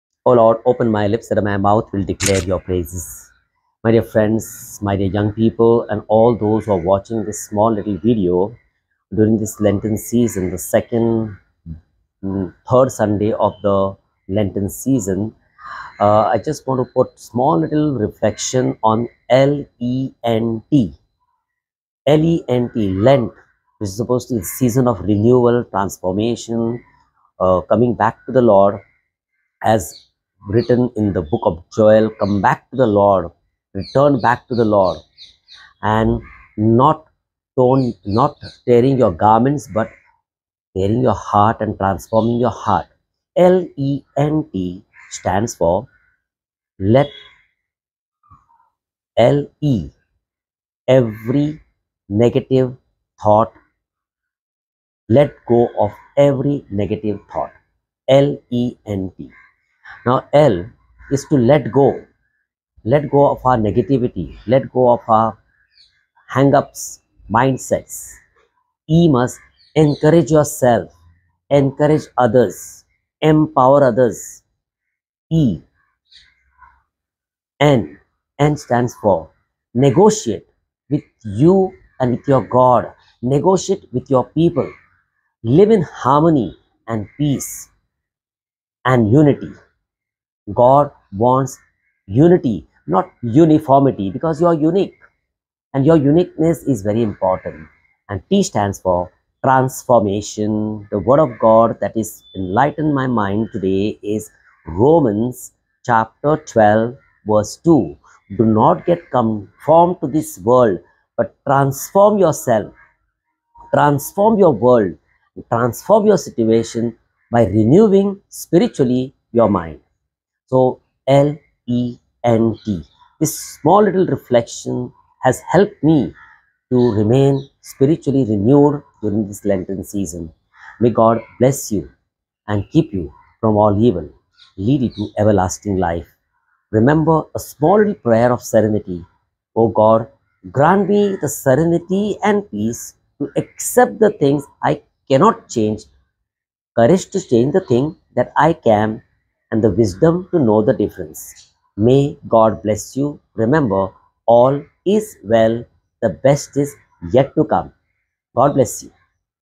Meaning of Lent. L E N T
Meaning of Lent.

My friends as we together go along the Lenten Season let's know whether we really are knowing the meaning of the word 'Lent' .

The Meaning of Lent is:

L -   Let go of your
       Negativities.

E -   Encourage and
        Empower yourself
        and others.

N -   Negotiate with
        yourself and others
       for Peace, not Strife.

T -  Transformation of
       your heart and mind.
       Don't try to change
       others.

By following the above in True Spirit, we can be sure of receiving God's blessings that awaits us at the end of this Lenten Season in the name of His Son Our Lord Jesus Christ. Amen🙏